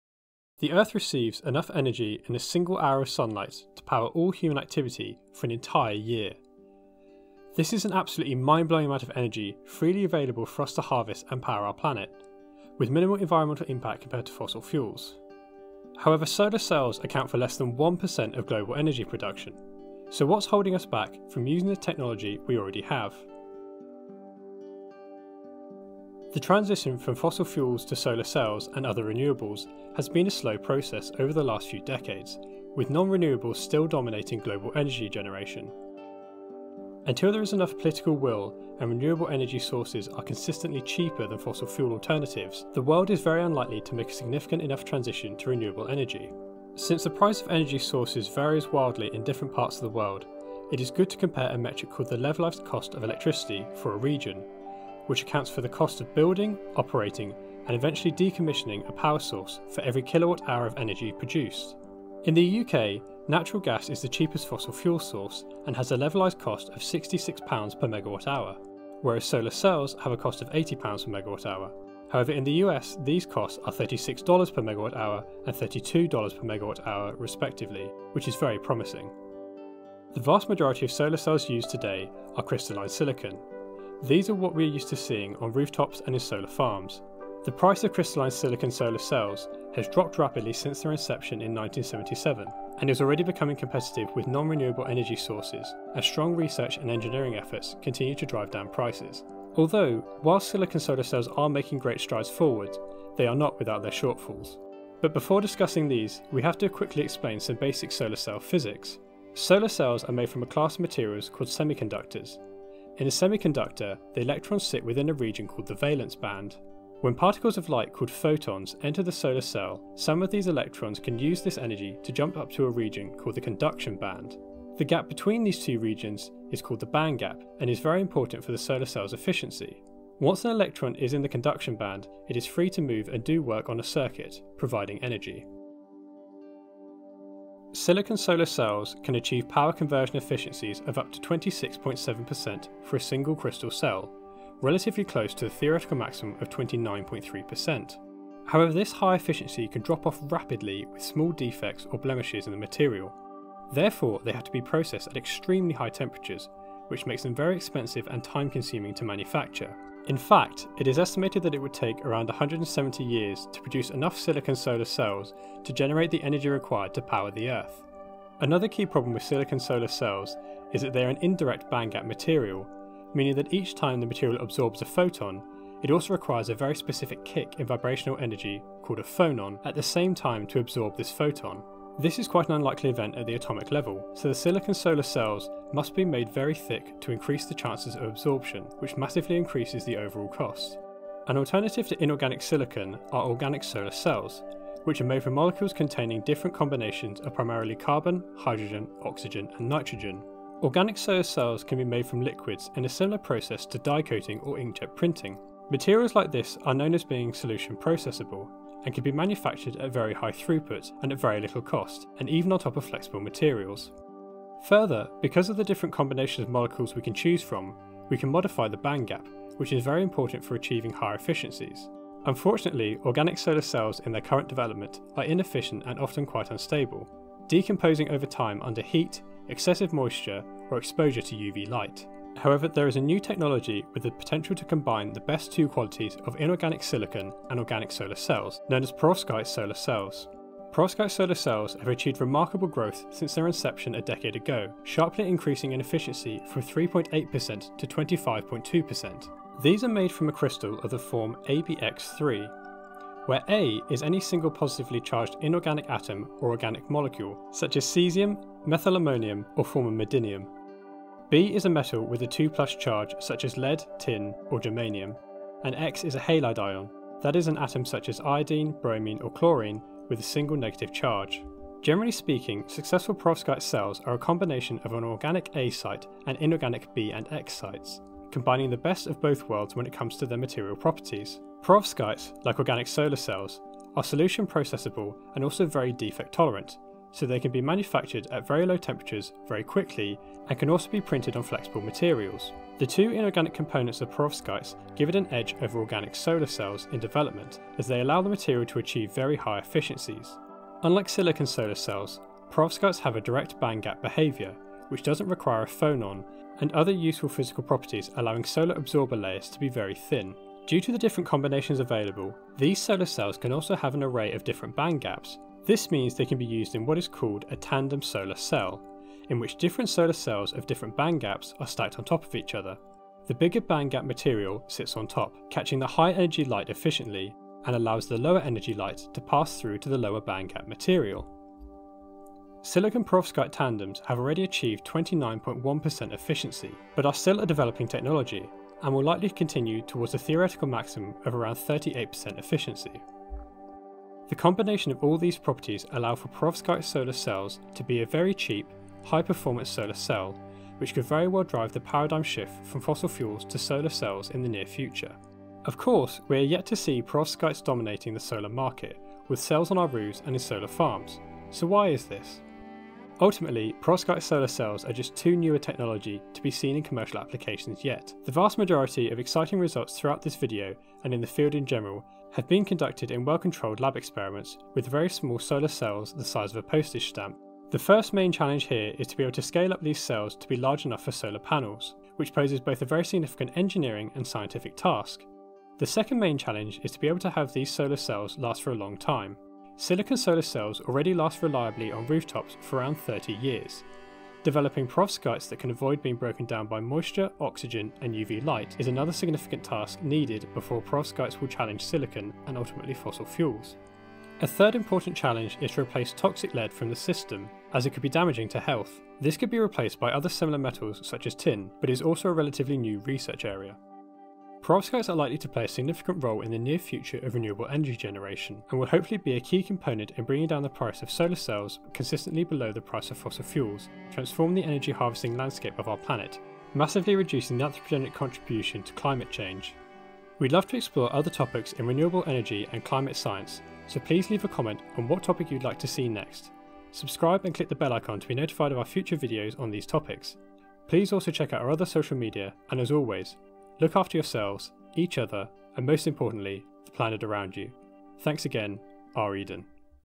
The Future of Solar Energy | Perovskite Solar Cells
Perovskite structures have the potential to revolutionise the solar PV industry, and have achieved remarkable growth since their inception a decade ago, very rapidly increasing in efficiency from 3.8% to 25.2%, near to their theoretical efficiency. Thus, the perovskite solar cell could revolutionize the future of solar energy advances. Perovskite solar cells are a new technology and still needs to be scaled up for commercial applications, but perovskites have the potential to revolutionise the energy harvesting market, with cheap, high efficiency PV technology in the near future. In this video, we'll explore perovskite solar panels and how they might energize our future.

We'd like to give a special thank you to Samuele from Bullaki ltd, for permission to use, and sending us, fantastic lab footage in the manufacturing of perovskite solar cells:

Scientific Video Protocols (Bullaki ltd), Back-Contact Perovskite Solar Cells | Prof. Sir Richard Friend et al. (University of Cambridge)

Other Sources:

SEM Images of OPV:
Cardinaletti, Ilaria, et al. "Toward bulk heterojunction polymer solar cells with thermally stable active layer morphology." Journal of Photonics for Energy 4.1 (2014): 040997.

Perovskite Review:
Roy, Priyanka, et al. "A review on perovskite solar cells: Evolution of architecture, fabrication techniques, commercialization issues and status." Solar Energy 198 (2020): 665-688.

References:

[3] - "Cost and Performance Characteristics of New Generating Technologies, Annual Energy Outlook 2019" (PDF). U.S. Energy Information Administration. 2019.
[5] - Chamberlain, G.A., 1983. Organic solar cells: A review. Solar cells, 8(1), pp.47-83.
[6] - Xiao, Z. and Yan, Y., 2017. Progress in theoretical study of metal halide perovskite solar cell materials. Advanced Energy Materials, 7(22), p.1701136.
[8] - Konstantakou, M. and Stergiopoulos, T., 2017. A critical review on tin halide perovskite solar cells. Journal of Materials Chemistry A, 5(23), pp.11518-11549.
[11] - Advanced Science News, Almost 40% conversion efficiency predicted in new perovskite solar cell,
[12] - Liu, Z., Sofia, S.E., Laine, H.S., Woodhouse, M., Wieghold, S., Peters, I.M. and Buonassisi, T., 2020. Revisiting thin silicon for photovoltaics: a technoeconomic perspective. Energy & Environmental Science.

00:00 - Introduction
01:46 - Some Basic Solar Cell Physics
02:17 - Silicon
03:12 - Organic Solar Cells
03:52 - Perovskite Solar Cells
05:18 - Tandem Solar Cells
06:02 - The Future of Perovskites
07:13 - Outro